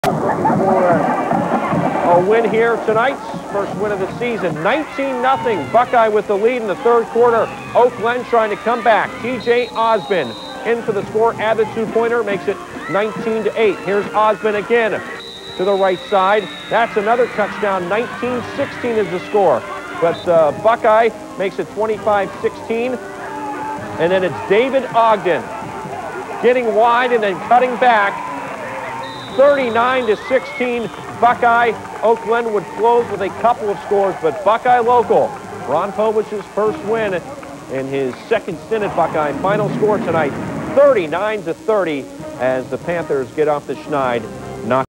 OVAC football - 2005 - Oak Glen v. Buckeye Local
2005 - Oak Glen Golden Bears v. Buckeye Local Panthers at World War II Memorial Stadium, Yorkville, Ohio.